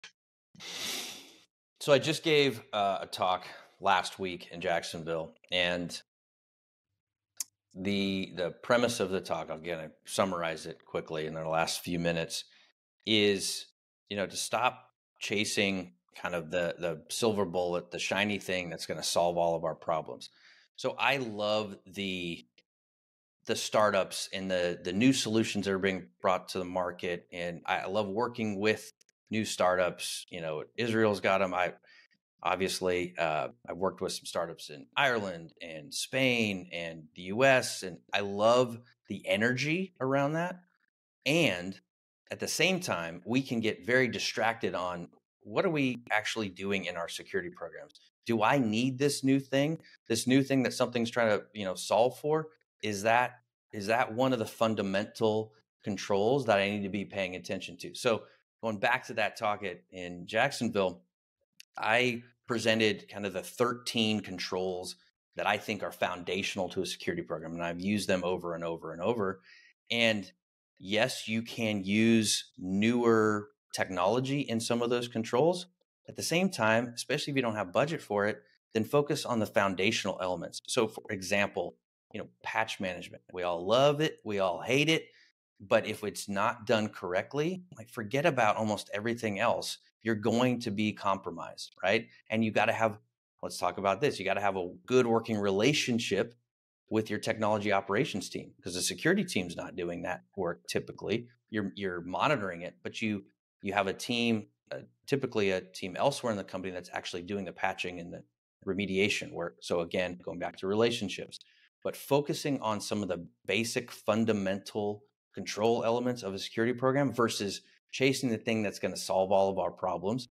Stop Chasing Silver Bullets And Focus On Cybersecurity Basics! 😎
In a world buzzing with shiny new technologies and startups promising to be the silver bullet for cybersecurity issues, it's crucial to remember the power of fundamentals. This insightful presentation from Jacksonville dives deep into why foundational controls are the bedrock of a solid security program. Instead of getting sidetracked by the newest tech trends, focus on solidifying your approach with essential practices such as effective patch management and building strong relationships across teams.

It's time to prioritise what truly matters and ensure your security strategy is robust before getting tempted by the allure of the next big tech solution!